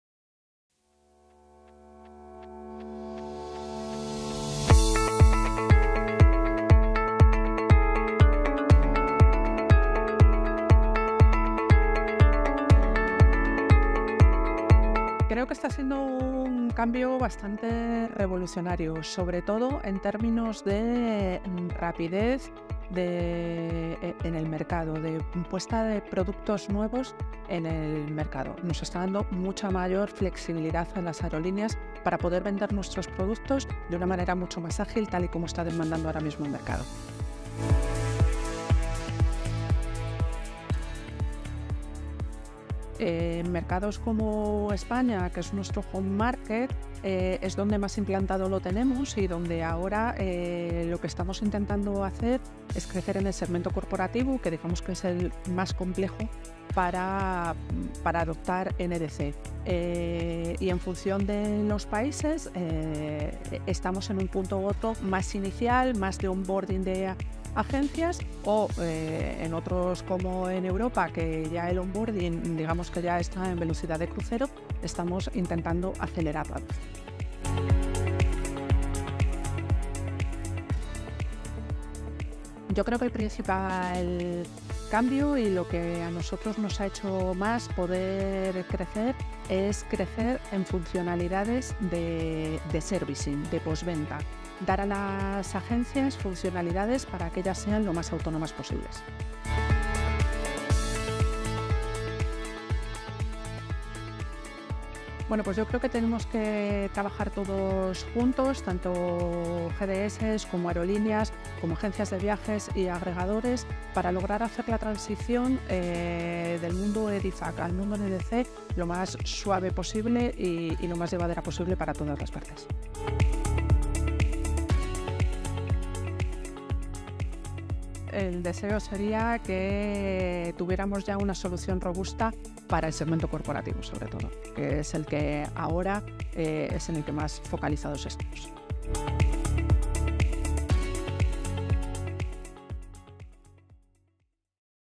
Industry perspectives on NDC: Why NDC is a high priority for Iberia and how they’re onboarding (ES)
Iberia’s NDC Onboarding & Operations Manager, Paloma Sanz de Andino, talks about how NDC is changing the industry and where they stand on their NDC journey, with the progress made since the beginning.